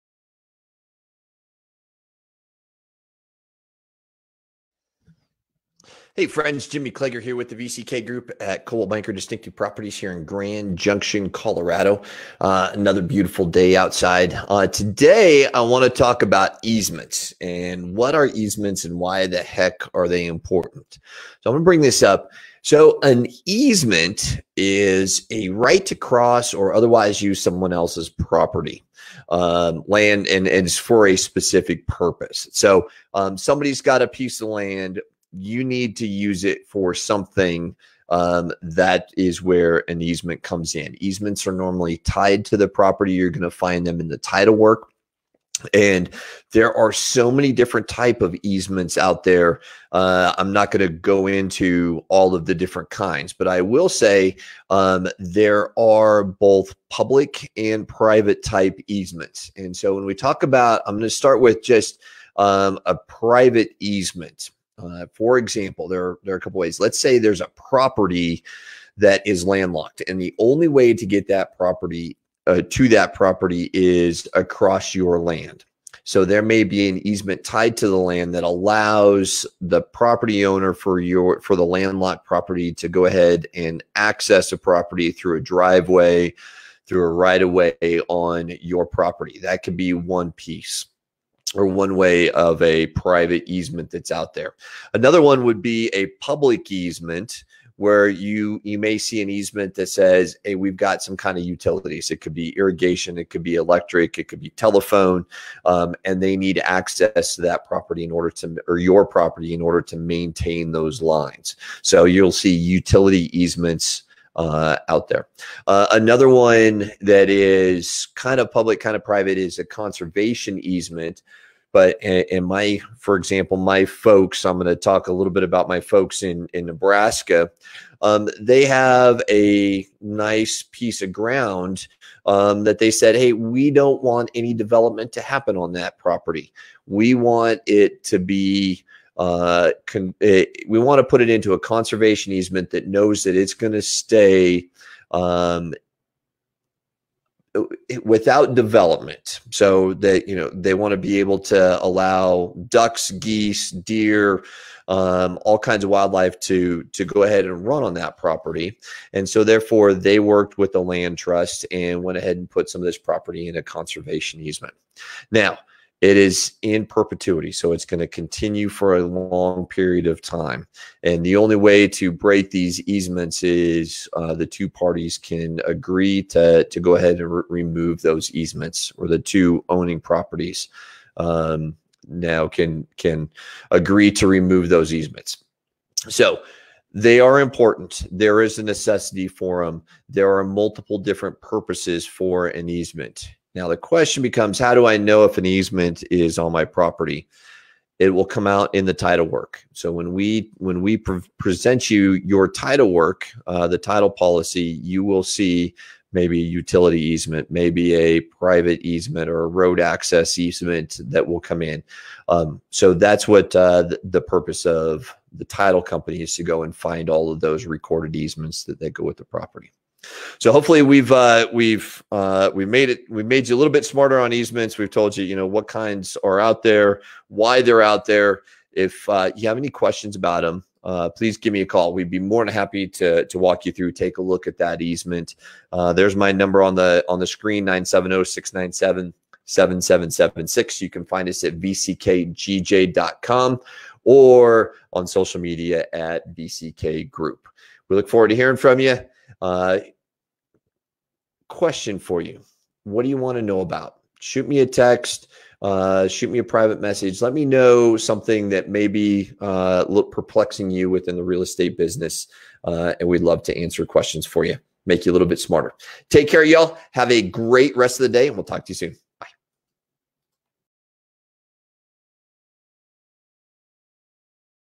What is an easement and do you need one?
Jimmy explains property easements and whether or not you might need one.